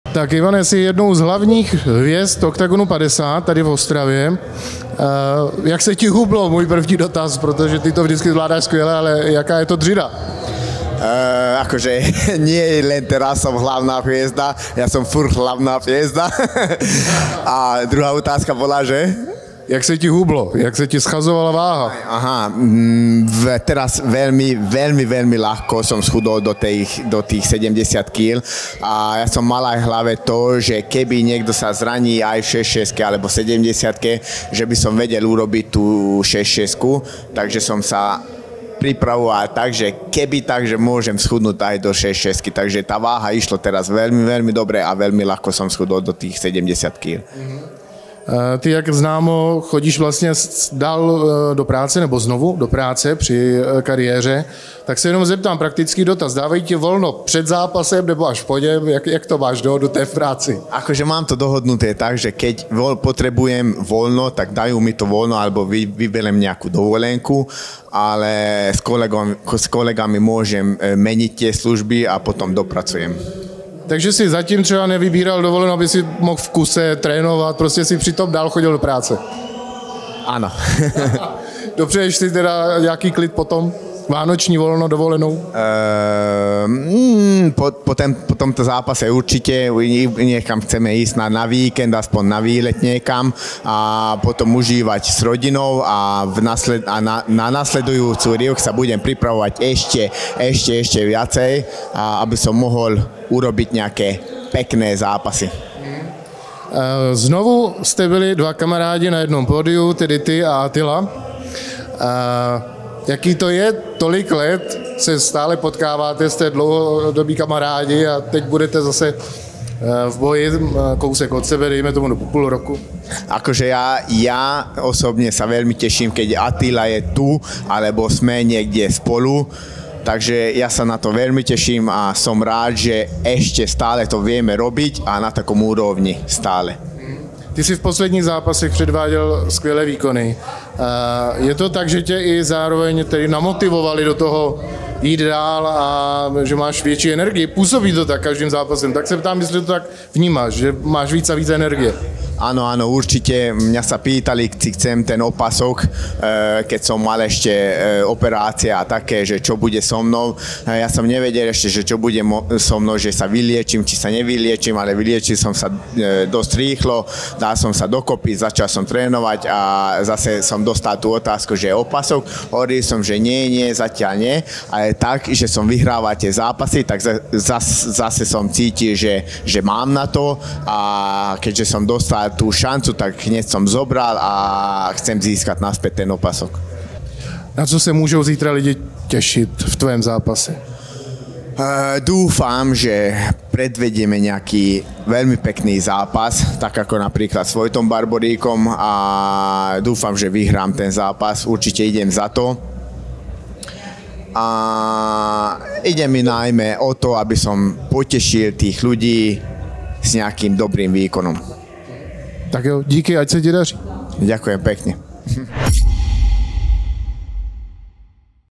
Předzápasový Rozhovor s Ivanem Buchingerem na Oktagonu 50
Fight Cast vyrazil na turnaj Oktagon 50 do Ostravy, kde se sešly ty největší hvězdy organizace. Nechybí Karlos Vémola, Attila Végh, Ivan Buchinger a další. OKTAGON MMA chce totiž jubilejním turnajem pořádně oslavit svou obdivuhodnou cestu. A tým Fight Castu vám chce nabídnout to nejlepší ze zákulisí celé akce. Oktagon se navíc vrátil do rázovité Ostravy, kde si náš podcast vybudoval provizorní studio, takže vám brzy nabídneme další unikátní rozhovory se silnými osobnostmi bojové scény. Sledujte díky Fight Castu turnaj Oktagonu úplně jinak než doposud.